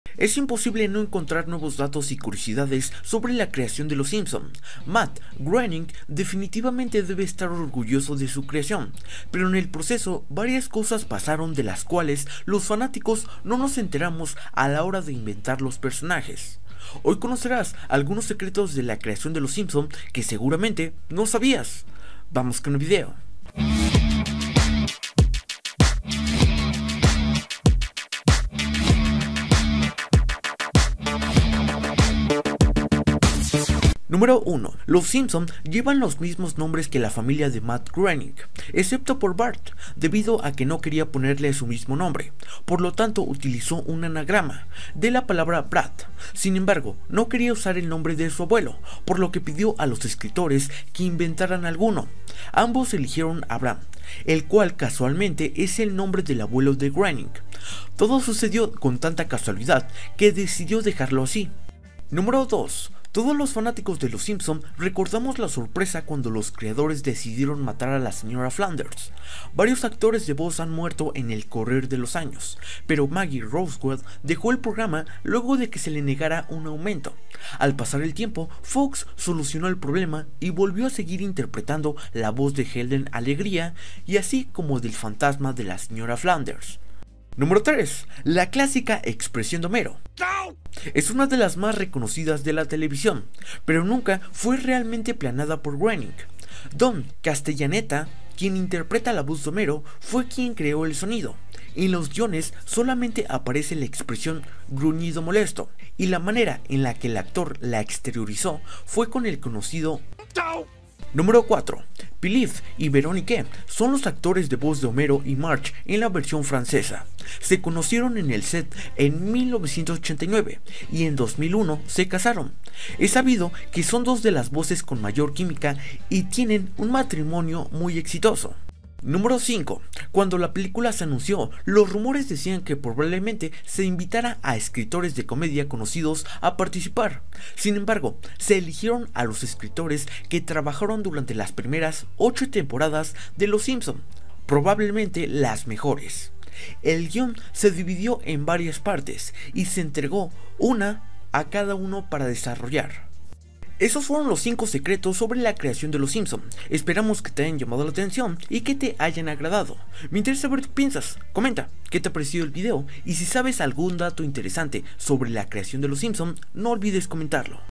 5 Secretos Sobre la Creación de Los Simpson
Es imposible no encontrar nuevos datos y curiosidades sobre la creación de Los Simpson, ya que en el proceso varias cosas pasaron de las cuales los fanáticos no nos enteramos.
Hoy conocerás algunos secretos de la creación de Los Simpson que seguramente no sabías.